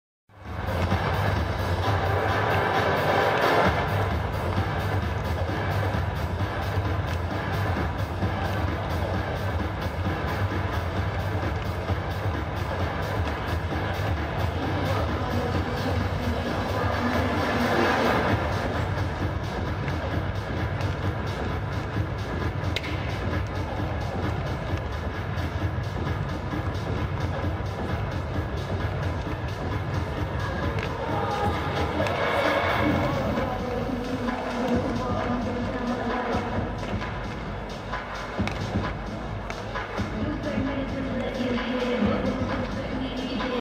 Winnipeg Jets goalie David Rittich warms up 1/17/23
Prior to the game vs. the Montreal Canadiens.